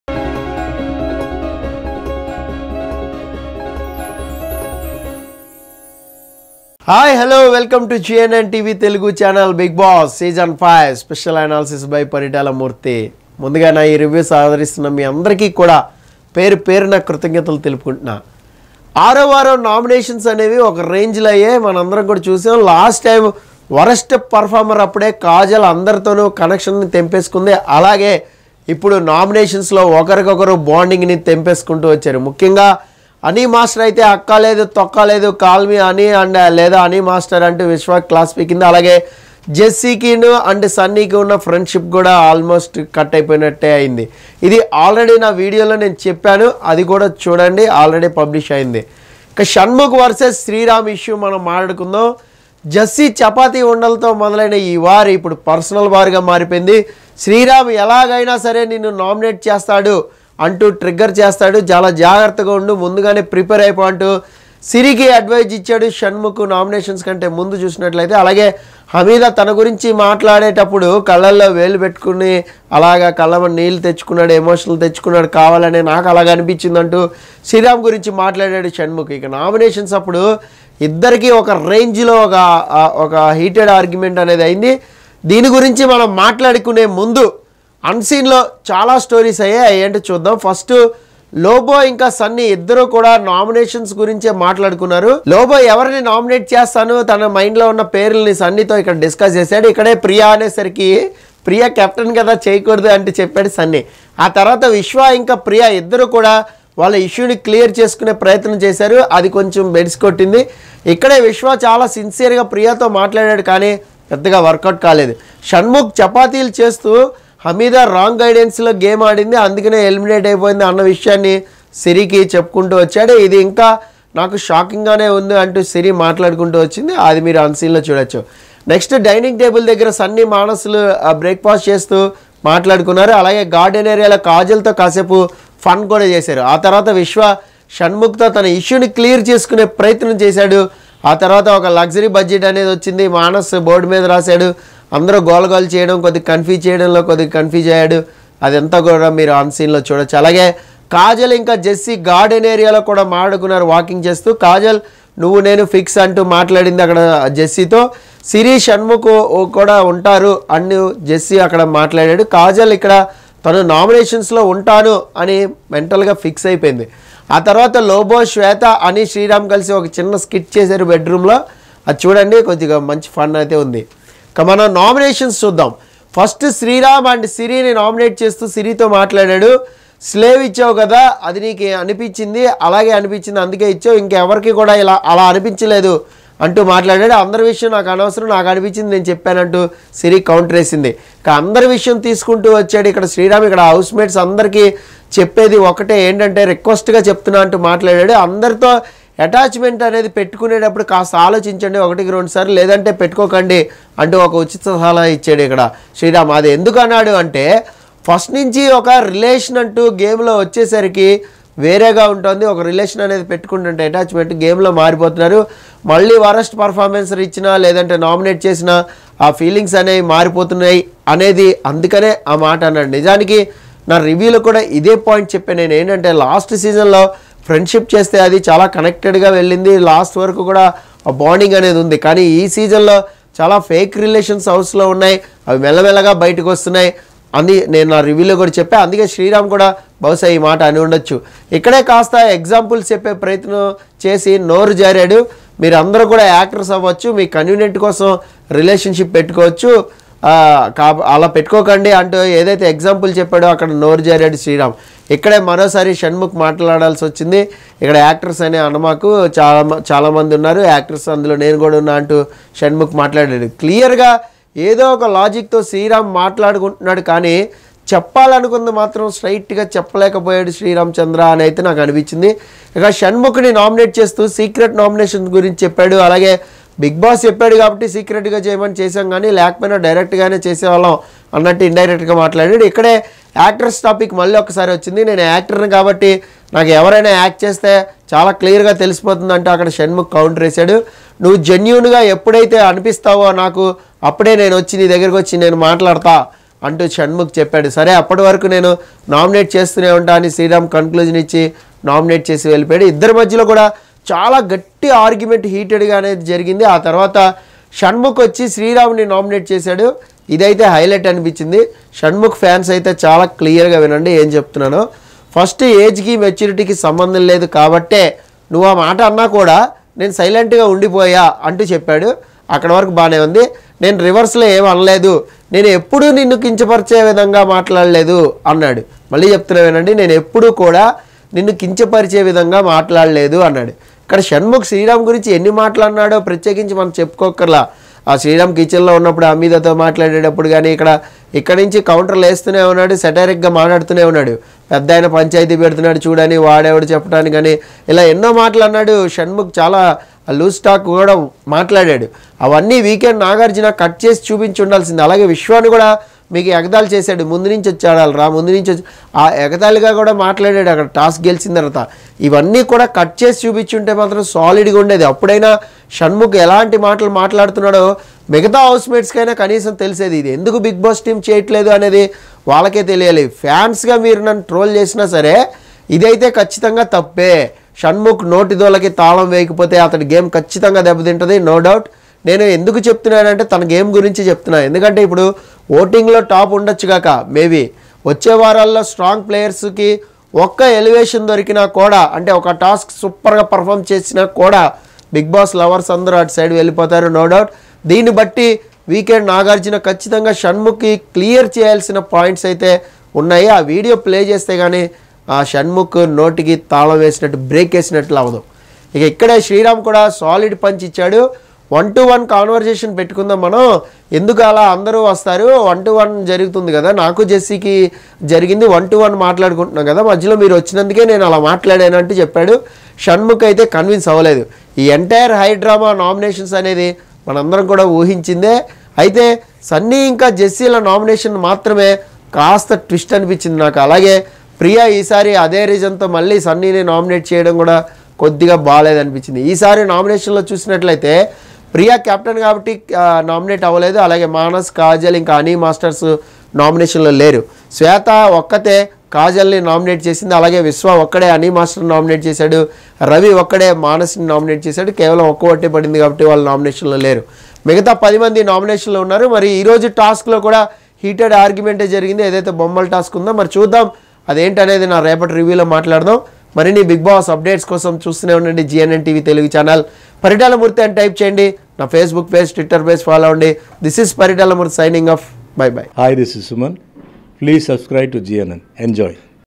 Shannu Vs Sriram l Nominations Fire🔥 Bigg Boss Telugu 5 Analysis By Paritala Murthy l GNN TV Telugu
nominations reasons on 6th week in bigg boss house l bigg boss telugu season 5 special analysis on nomination reasons shannu vs sriram